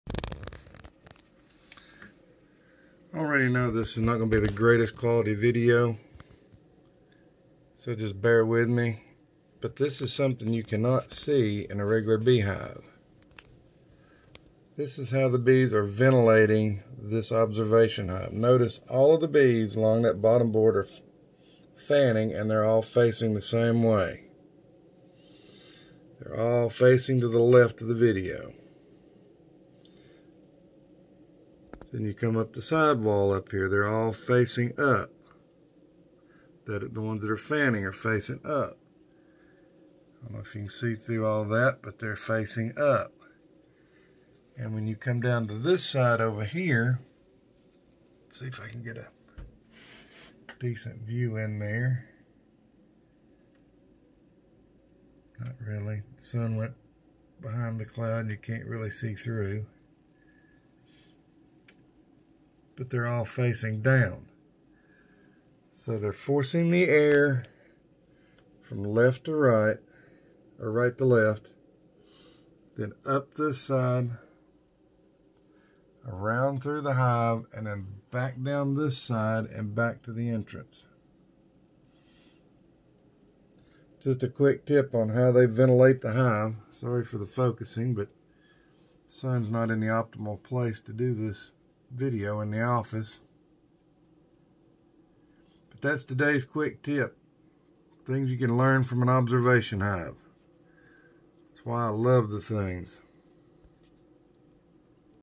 How Bees Ventilate a Hive
i had it right the first time they are ventilating the hive from left to right pulling air in and up by the entrance up then across the top and down the left side across the bottom and out the exit.